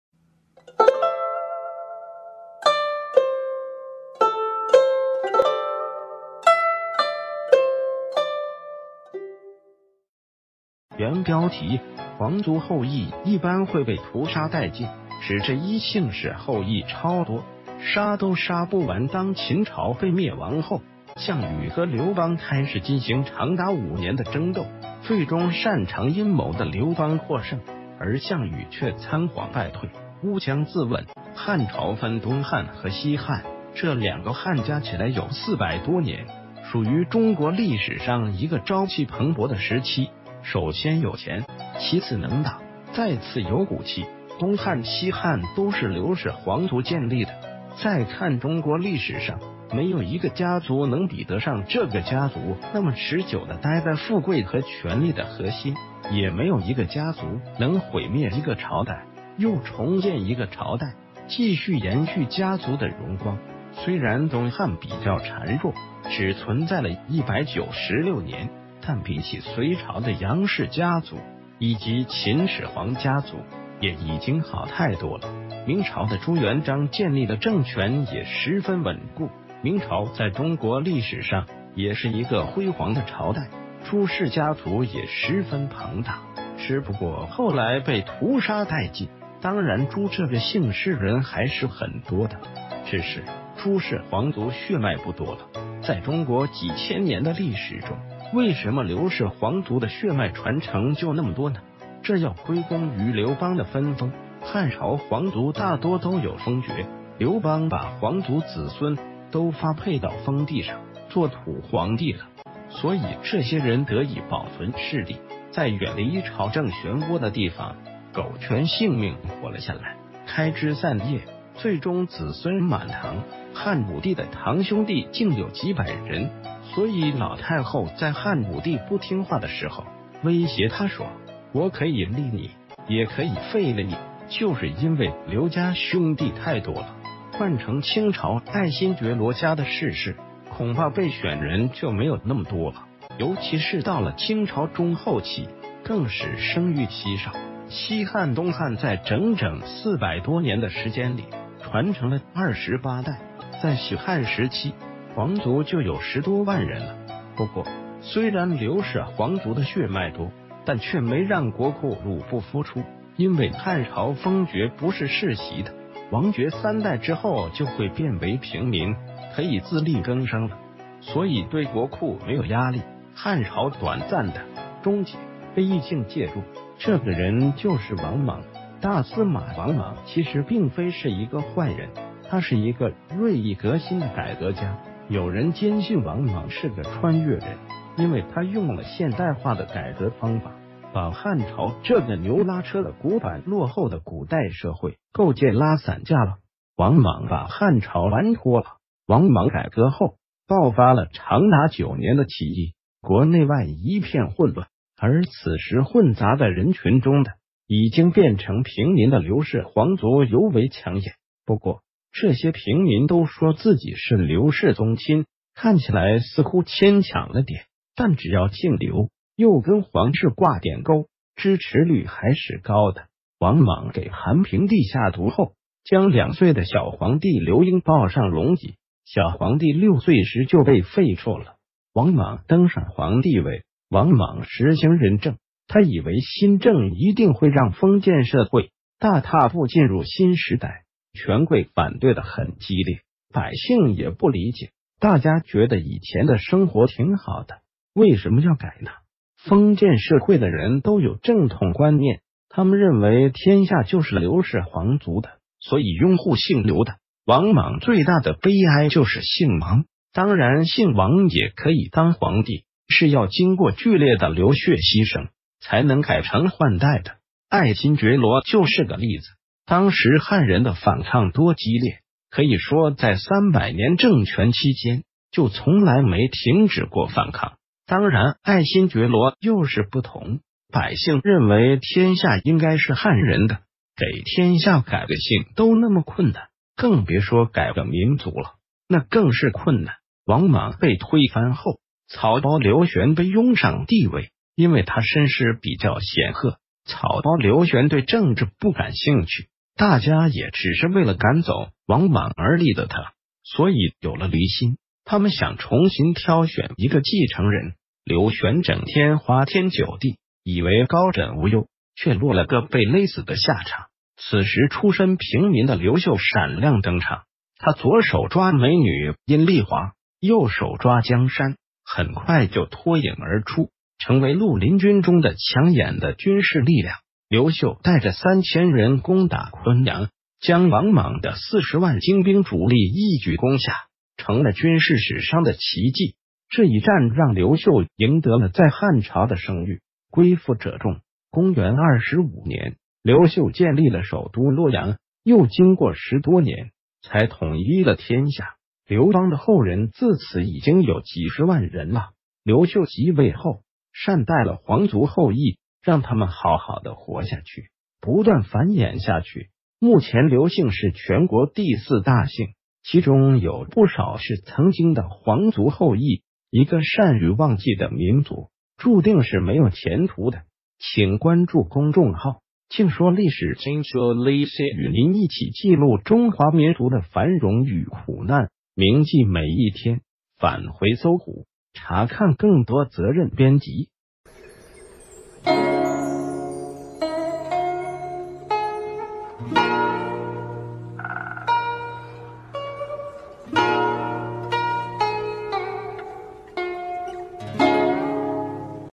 皇族后裔一般會被屠殺殆盡，只這一姓氏后裔超多，殺都殺不完_搜狐歷史_搜狐網
原標題：皇族后裔一般...